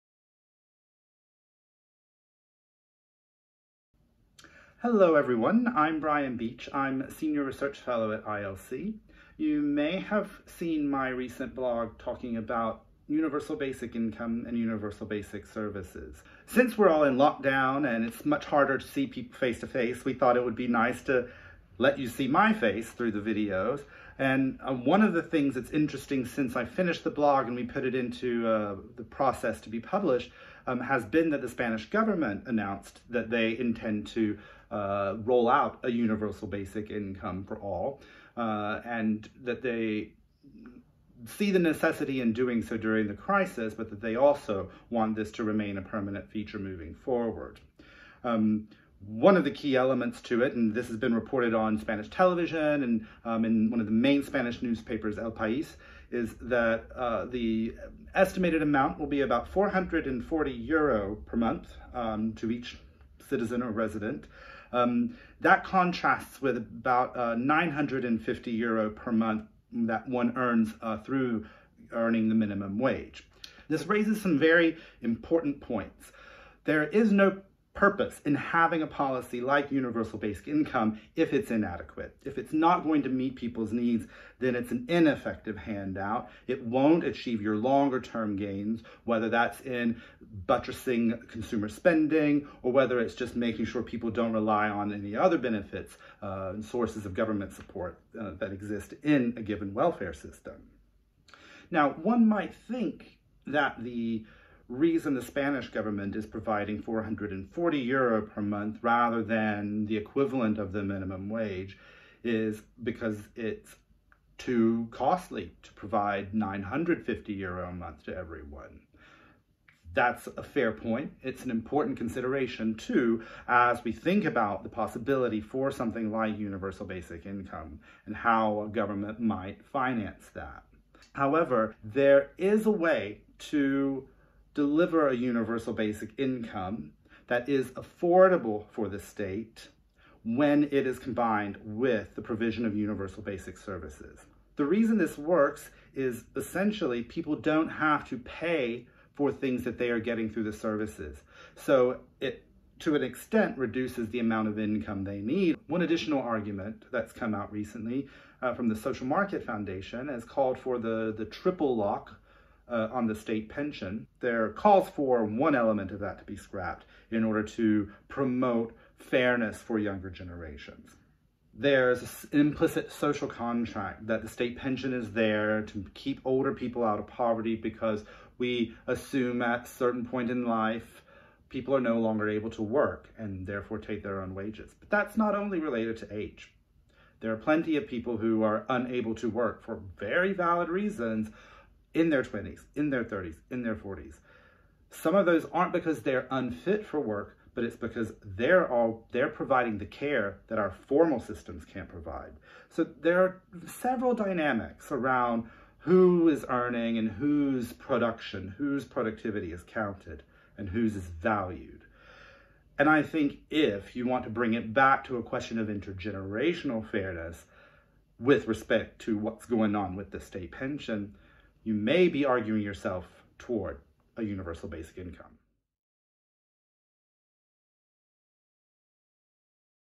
Dr Brian Beach discusses the potential of Universal Basic Income and Services in light of COVID-19
In this video, ILC Senior Research Fellow Dr Brian Beach discusses the potential of Universal Basic Income (UBI) and Services (UBS) in light of the current coronavirus crisis.